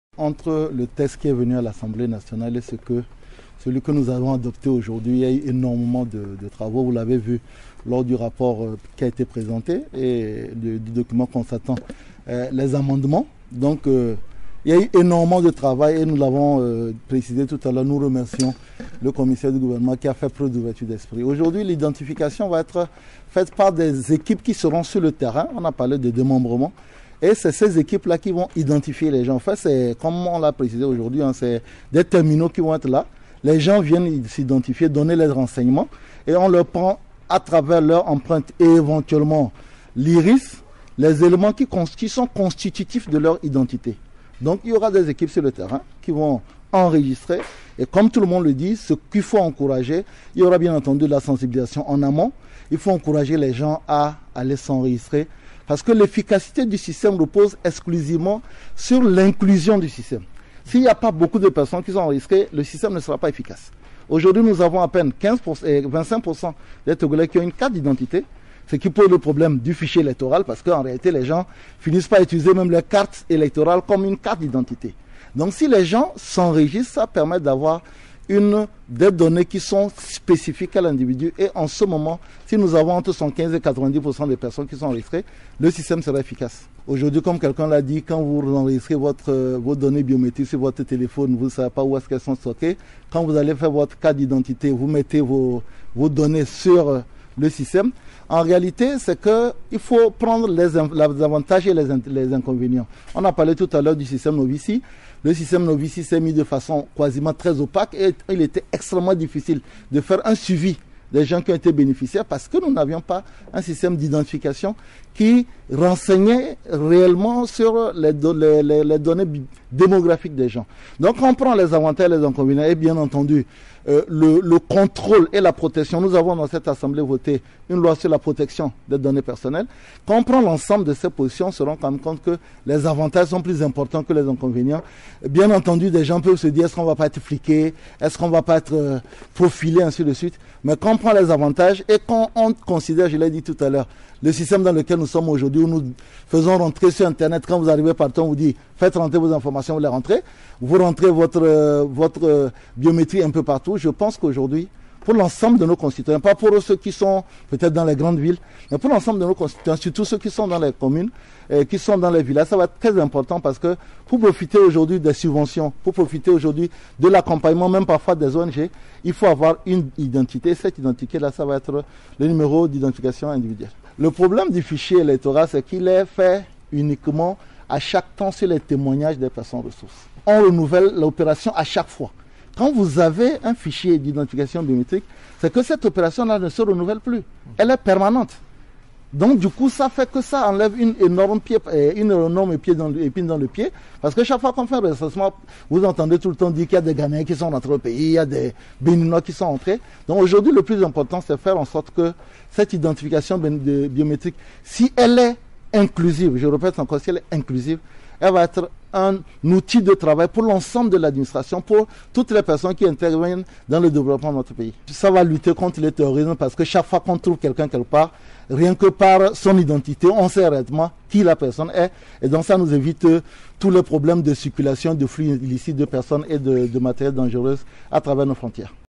Les avantages de la loi sur l'identification biométrique des personnes physiques (Gerry TAAMA)
Les avantages de la loi sur l'identification biométrique des personnes physiques dans le quotidien des Togolais (Gerry TAAMA)